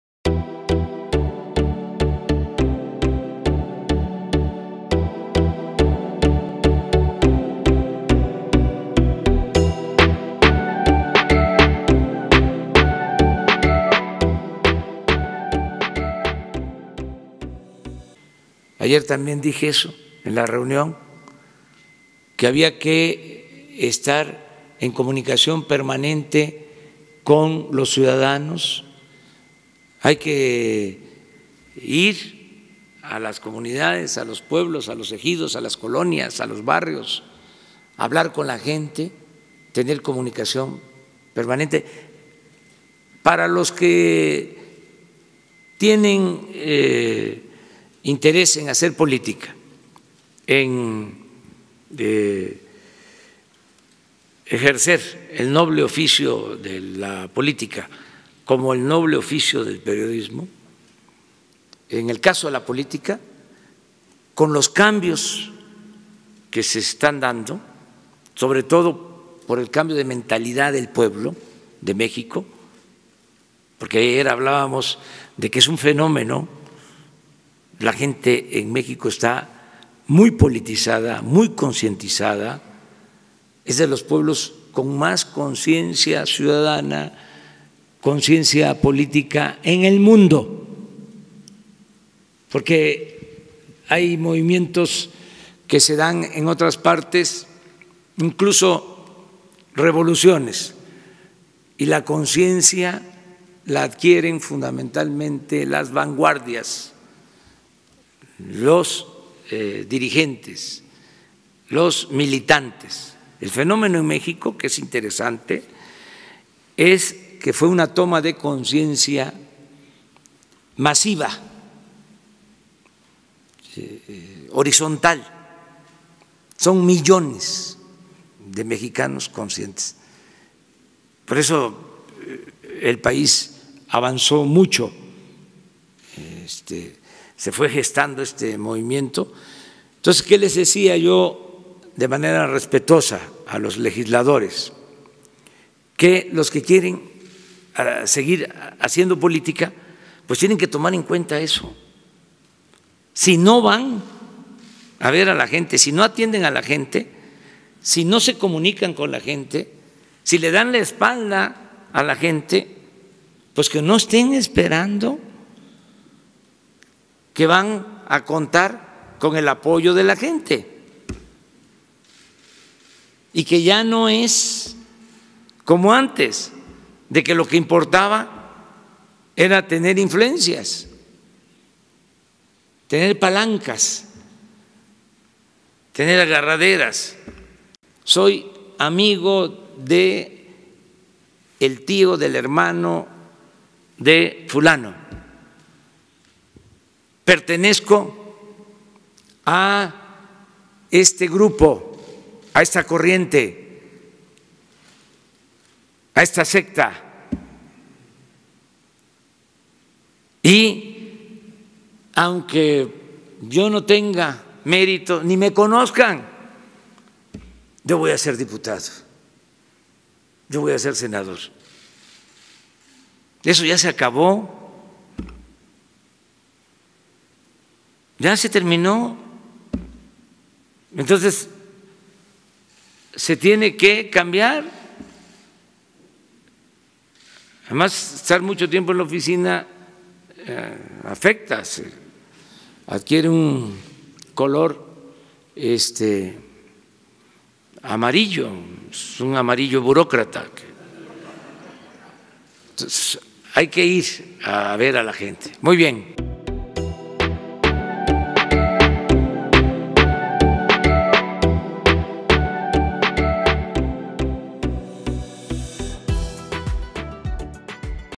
CONSEJO DE AMLO a todos los que quieren ejercer el NOBLE OFICIO de la POLÍTICA.
En este video el Presidente de México Lic. Andrés Manuel López Obrador manda un claro mensaje a todas aquellas personas que quieran hacer política; "ya no es como antes (influencias, palancas, etc.), ahora lo que importa es estar en contacto con la gente, ir con la gente": AMLO